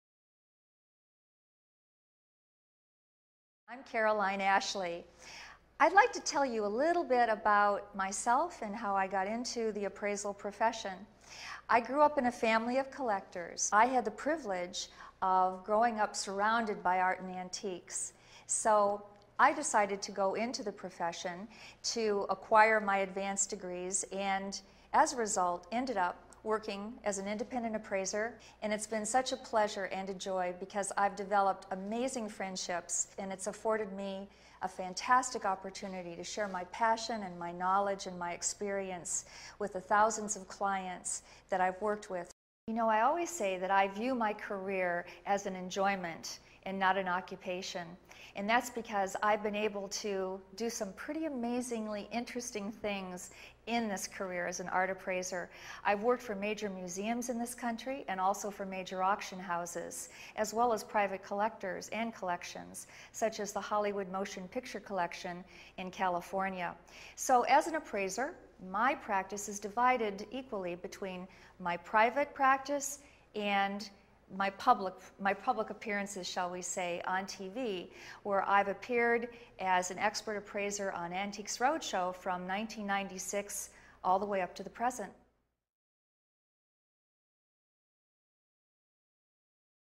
Caroline Ashleigh & Associates on Her Career in Appraising Fine Art & Antiques
Caroline Ashleigh & Associates, appraiser of fine art and antiques discusses her career in appraising fine art & antiques. Caroline Ashleigh is a high profile appraiser who has appeared on Antiques Roadshow and HGTV. To learn more, visit:

Watch for Caroline Ashleigh on Antiques Roadshow & HGTV